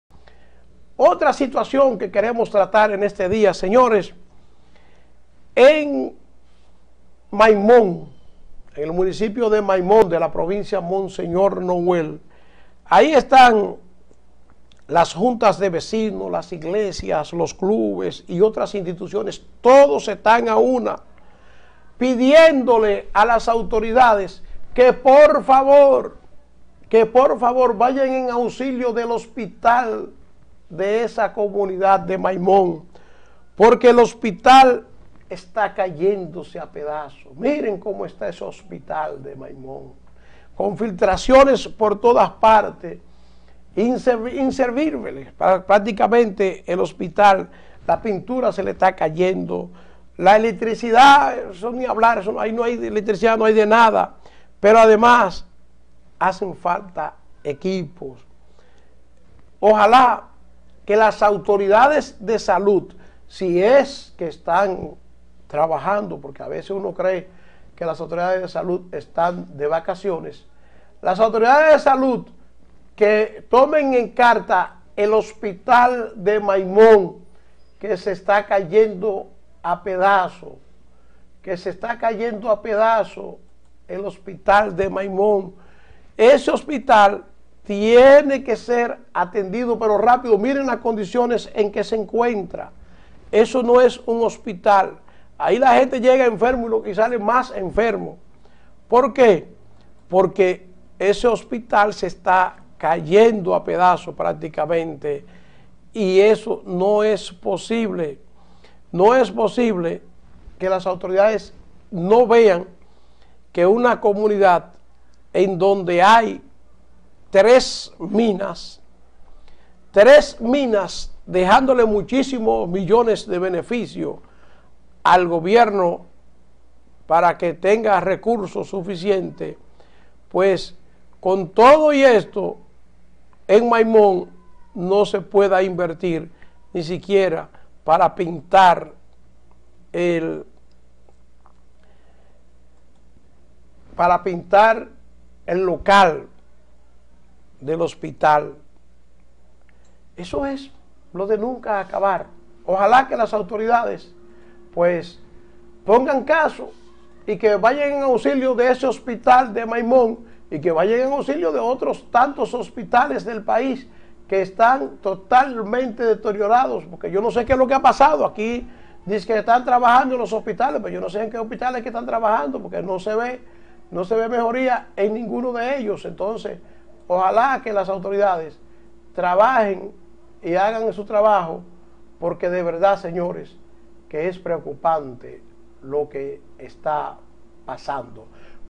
HOSPITAL DE MAIMON EN MAL ESTADO
Claman por ayuda para el hospital de Maimón que se cae a pesazos. mas detalles